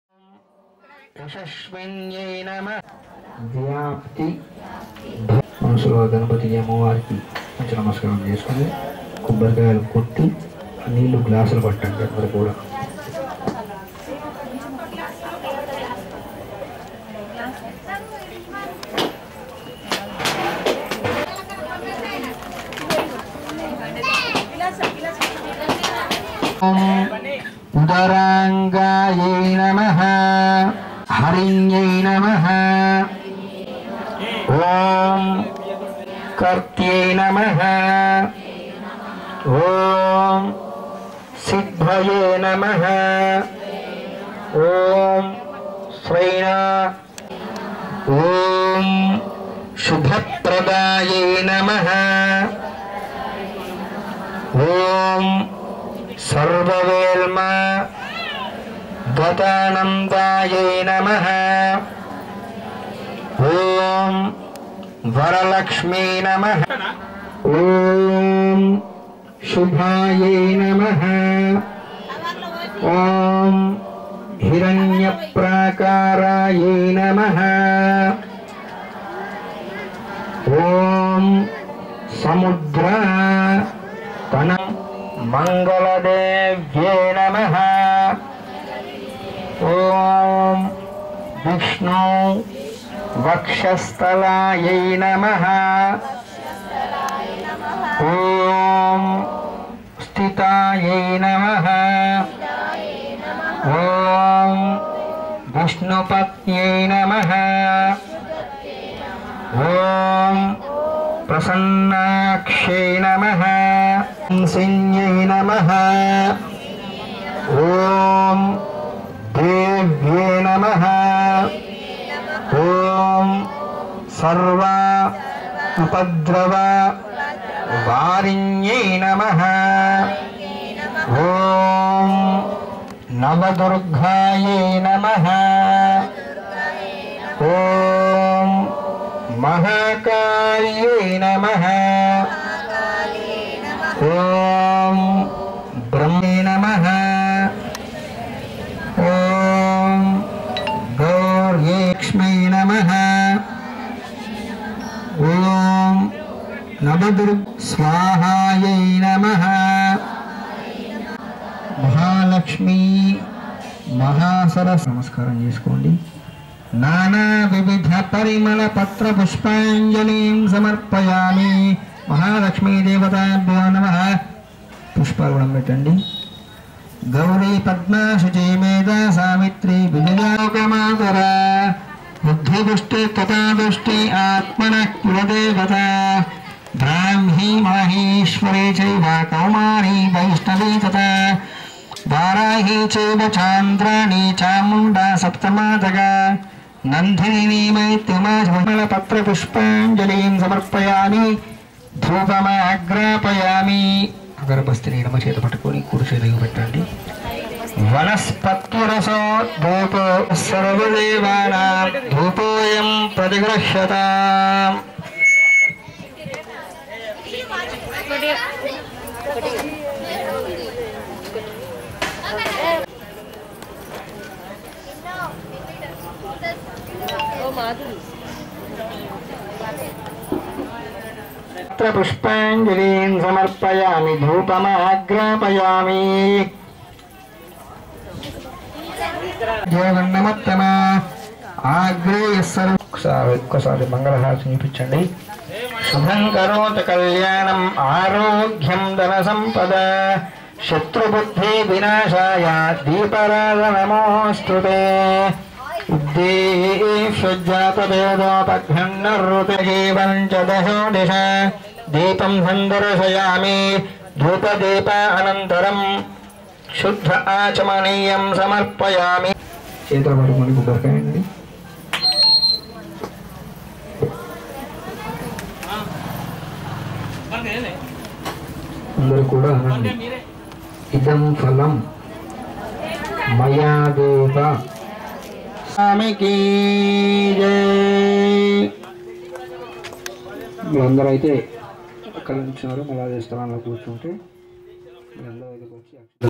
Sameera Channel News - Jagtial | 05.09.2019
జగిత్యాల పట్టణంలోని విద్యానగర్ వాటర్ ట్యాంక్ వద్దగల వినాయక మండపంలో కుంకుమ పూజలు